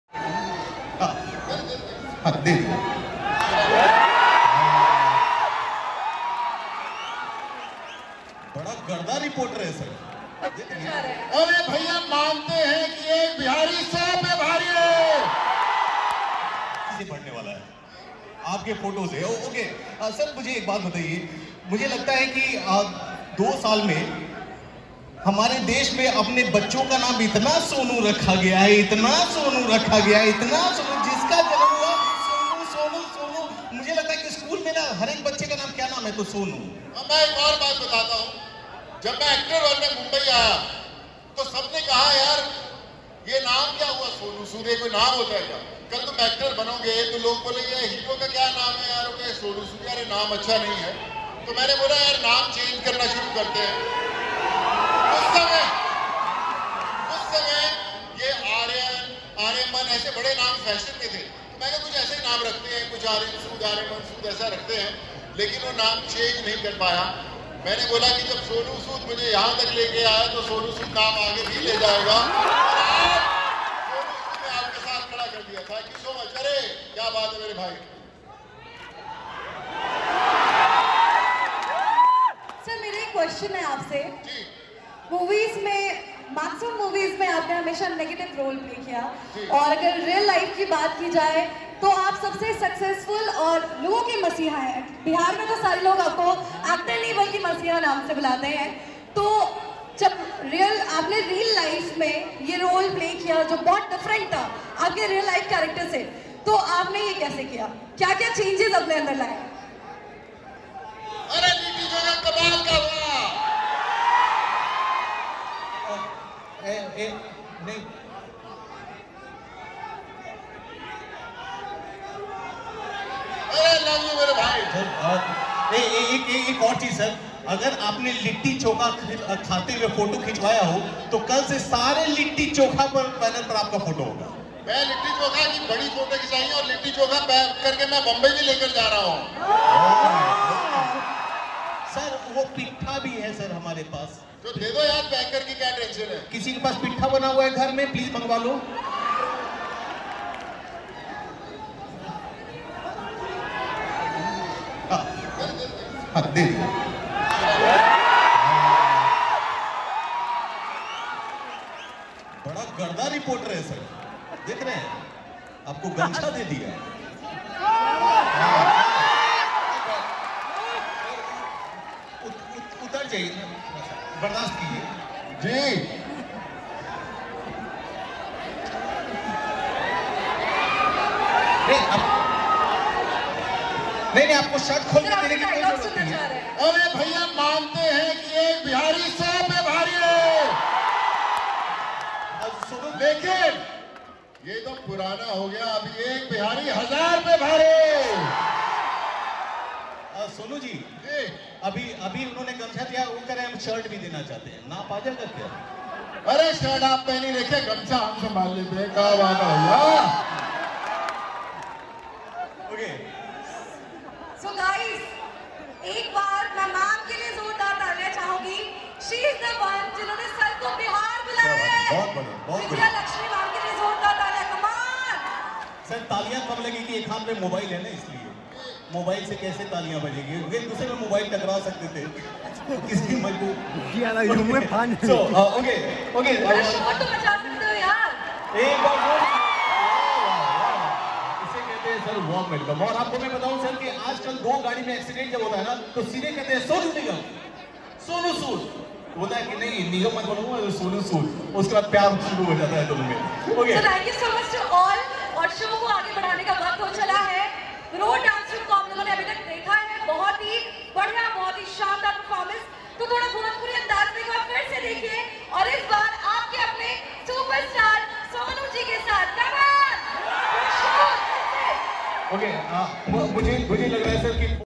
अचानक मंच के नीचे से Sonu Sood ने लिया गमछा, कहा एक बिहारी सब पर भारी |Sonu Sood Patna
bihar sonu sood